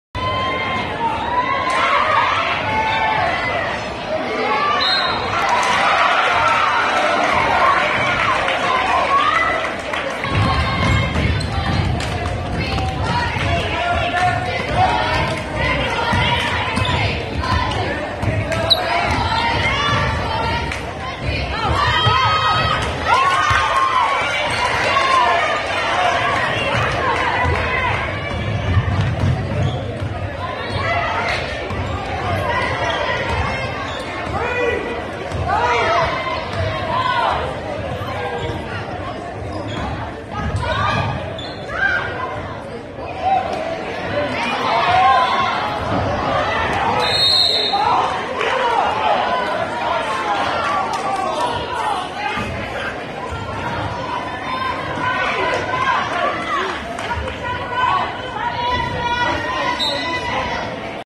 West Henderson vs. Madison girls
West hosted Madison on Nov. 22, 2017. Madison's girls won 77-59.